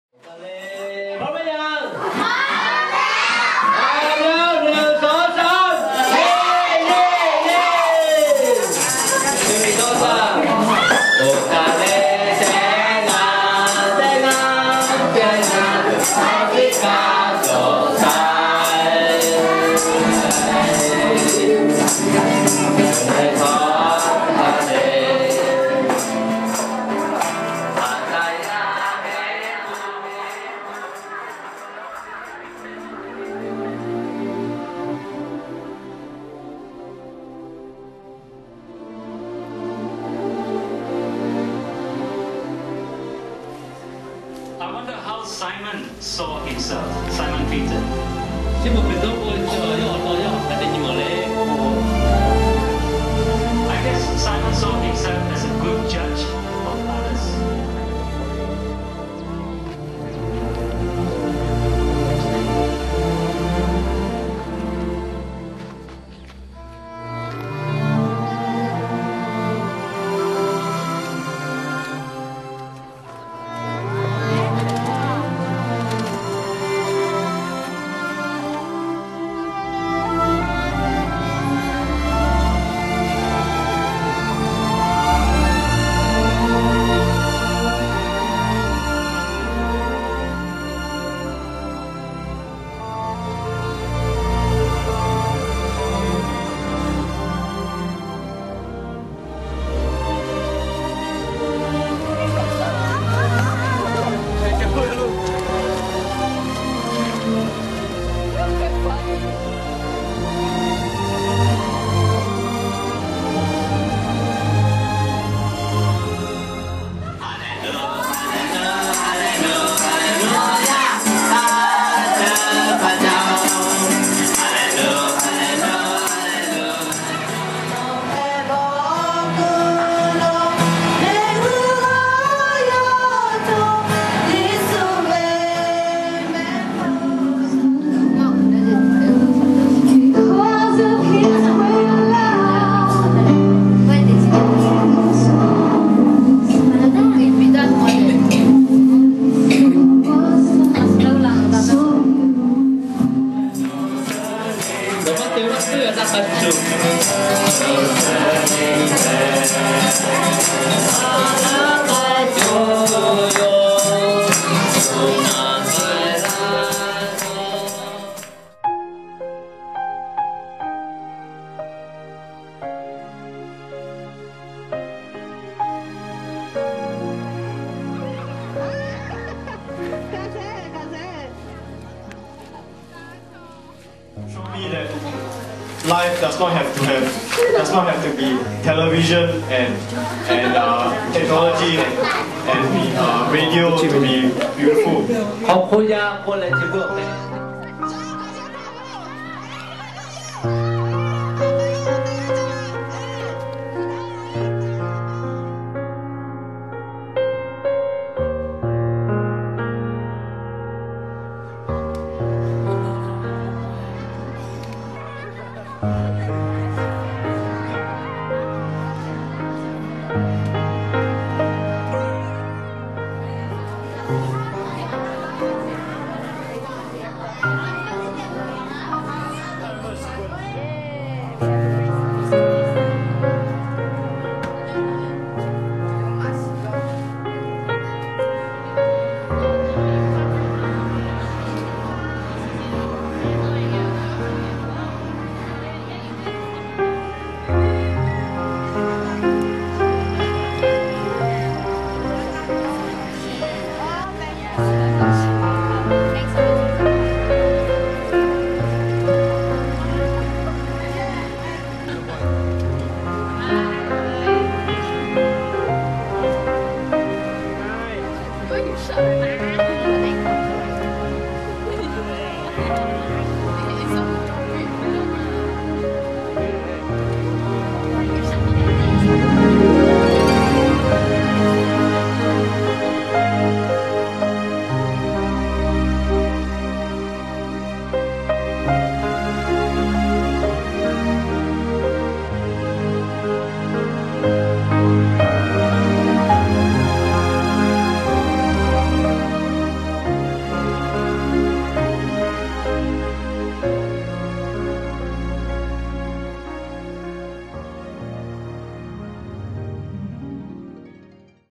Lahu Mission 2005 / Missions Sunday Video
Mission Trip to Huay Parrai, a Lahu village in Thailand.